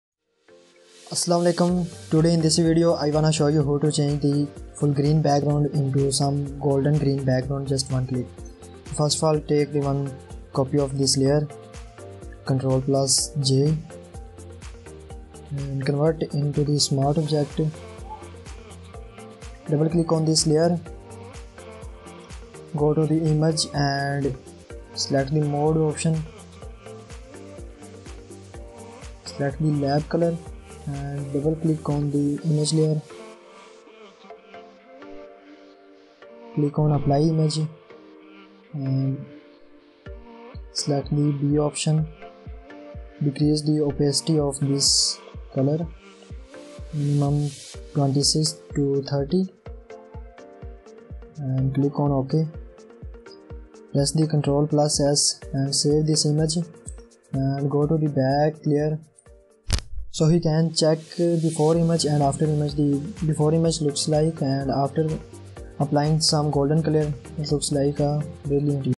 Apply Some Golden Color effect on background in just one Minute / Photoshop 2021 tutorial
In this video i wanna show you how to change or Apply Some Golden Color effect on background in just one Minute / Photoshop 2021 tutorial  Background Editing
if you follow my video you can change simple background to multi color background it's very working

 notifications button so you’re among the first to know when a new video comes out.